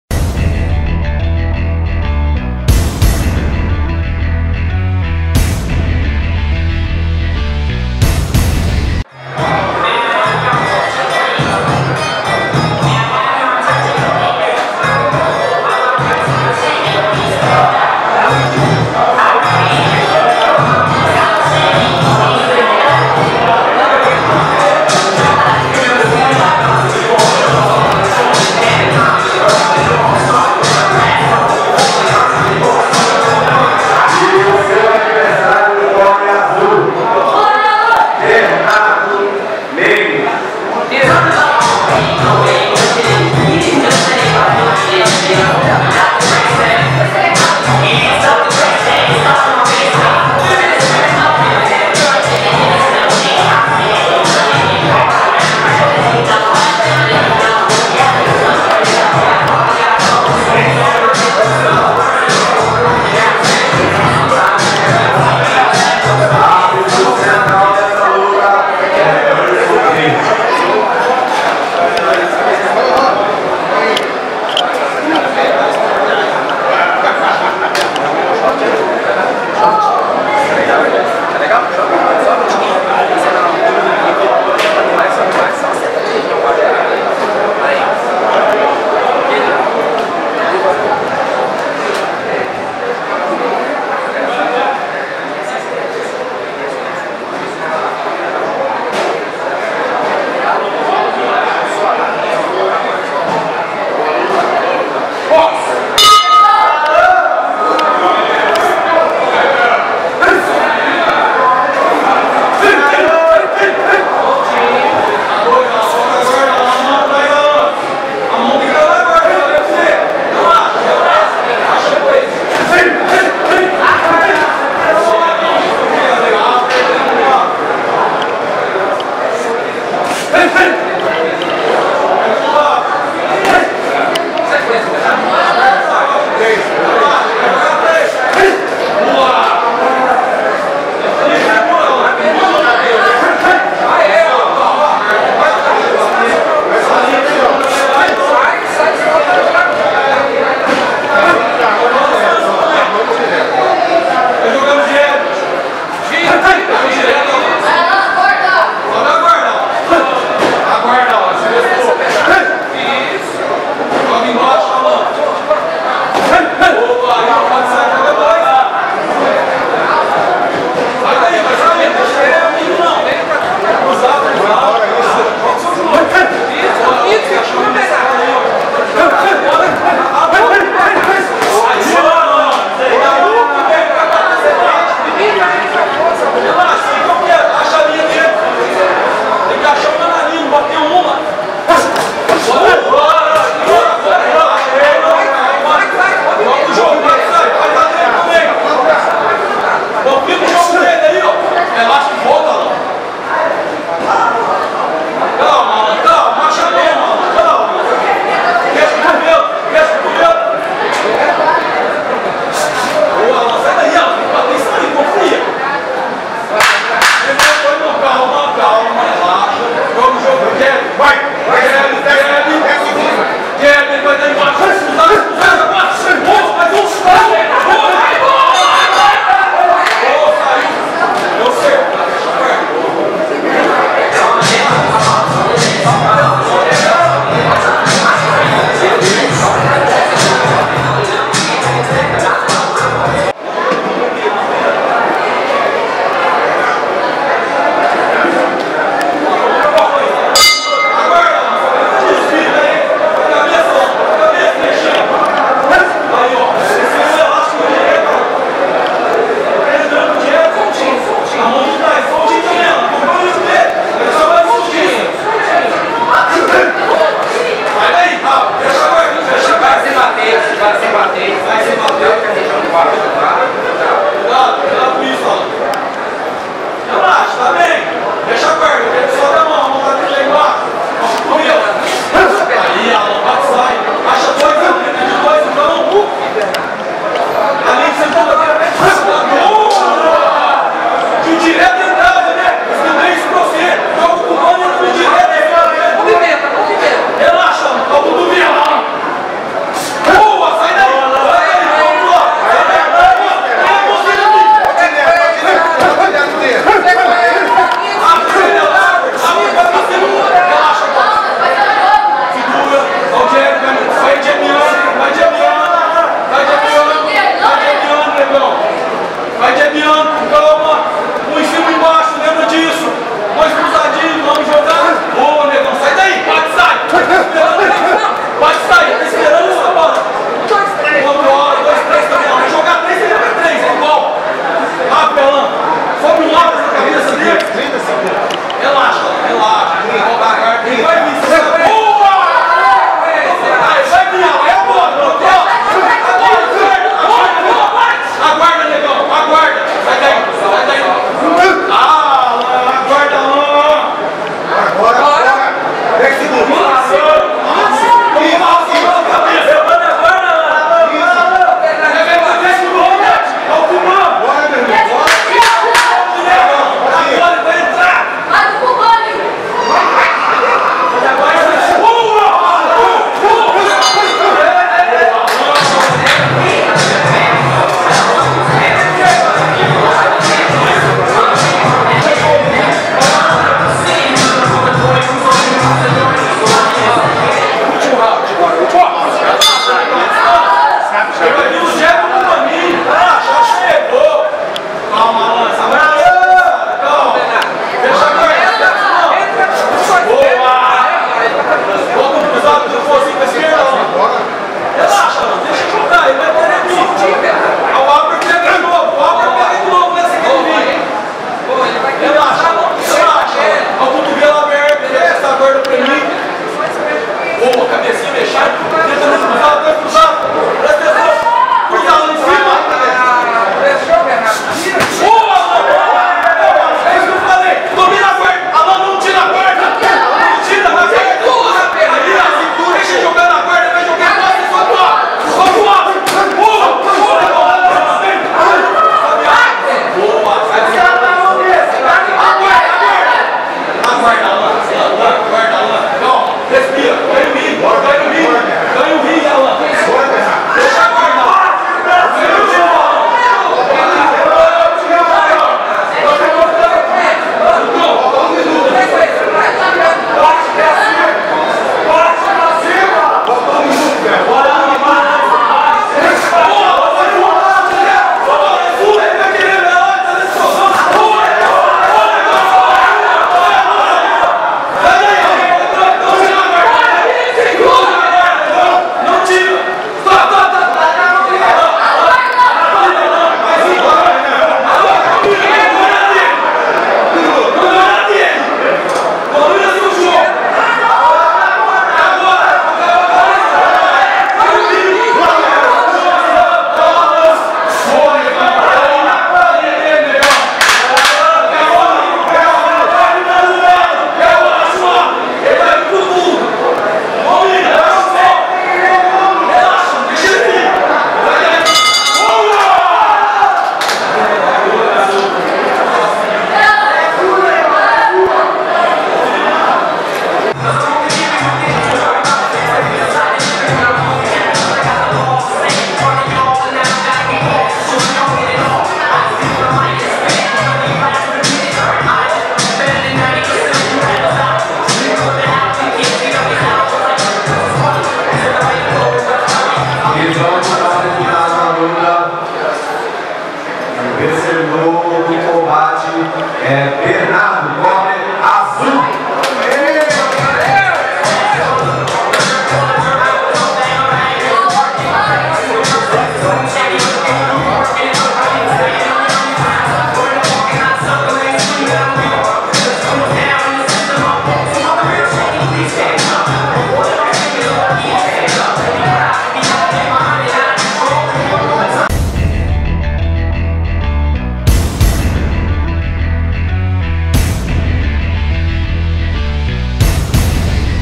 Bernardo vs Alan Delon - Copa Pedro Maciel
Em combate de Boxe amador até 81kg, Bernardo Menezes (Rolo Fight Team) venceu Alan Delon (Boxe Nova Geração) por pontos.